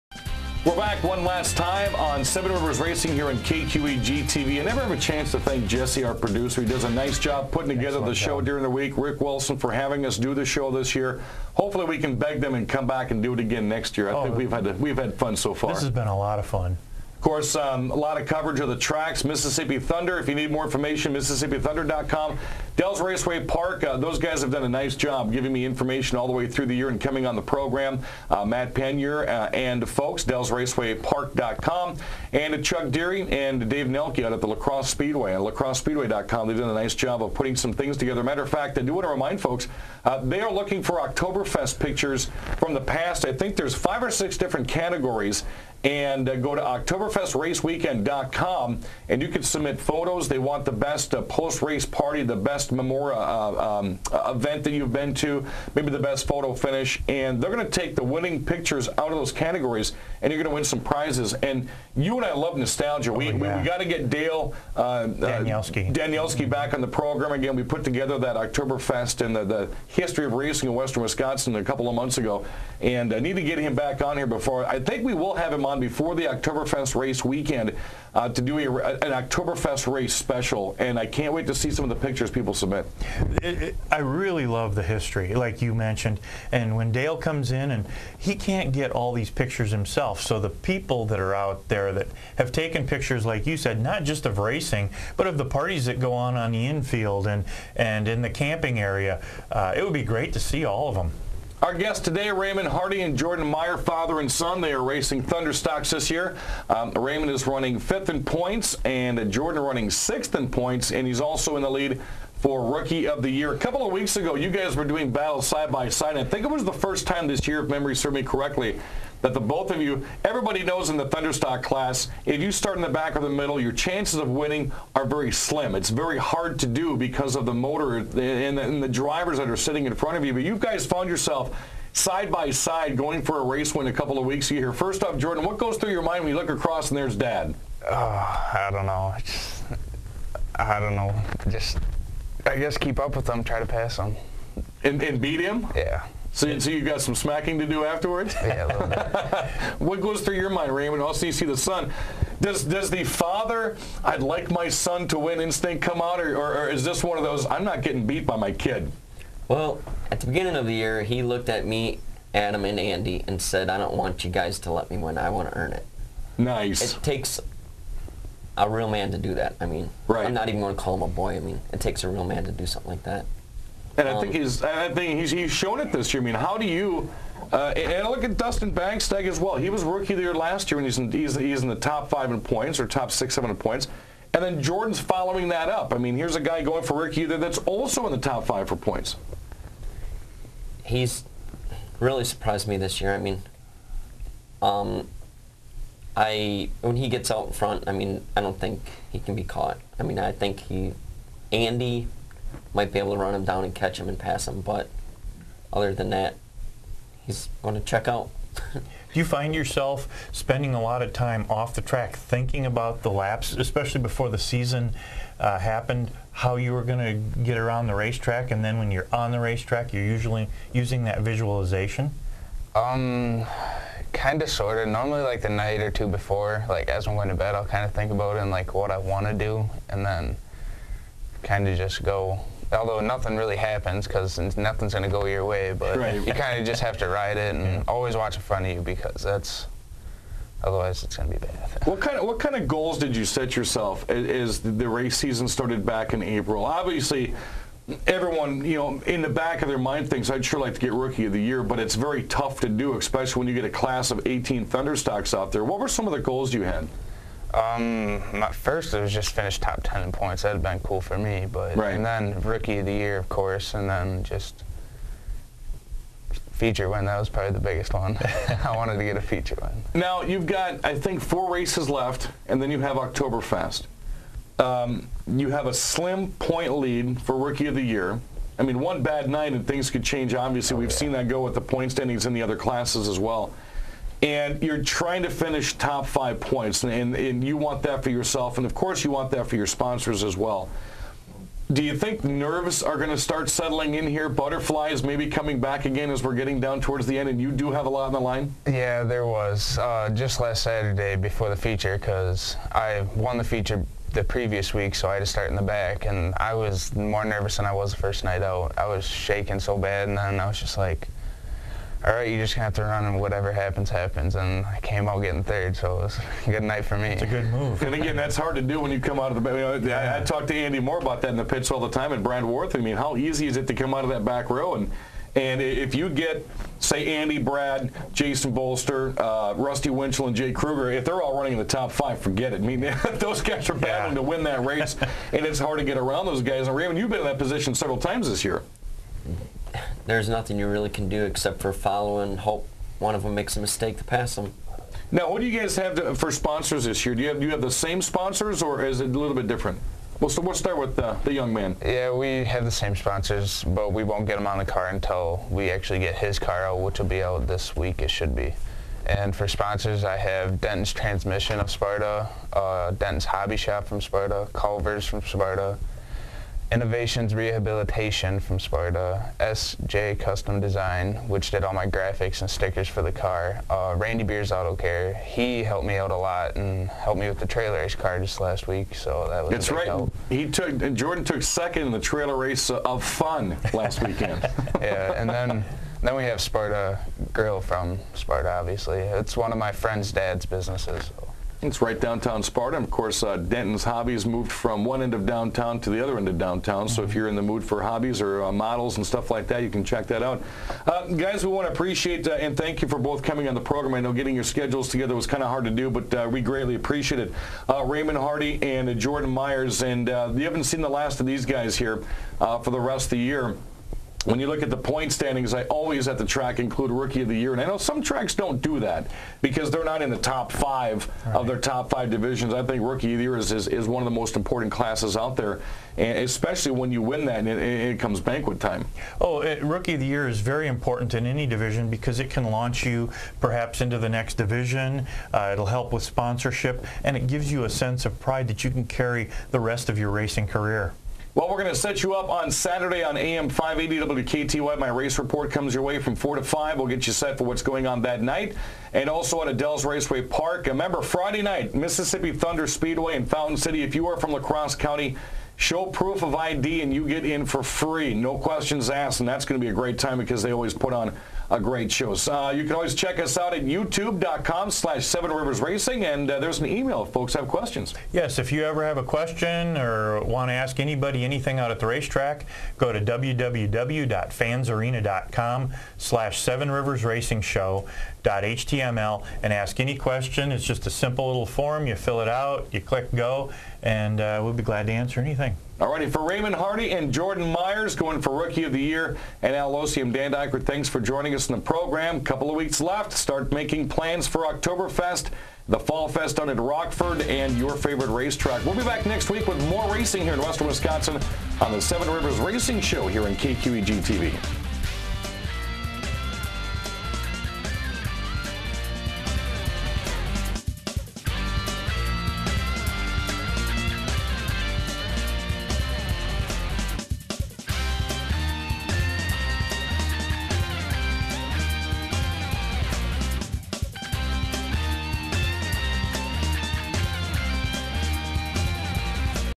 Seven Rivers Racing Aug 11th, 2011 pt 3 of 3
Al and Dan continue their discussion with the Father-Son duo of Raymond Hardy and Jordan Myers. Both men are in the top five in the Thunderstock division out at the La Crosse Fairgrounds Speedway, making things interesting around the dinner table!

Sponsors included Lost Lake Beer, Cary Heating and AC, Holmen American Legion and West Salem Pharmacy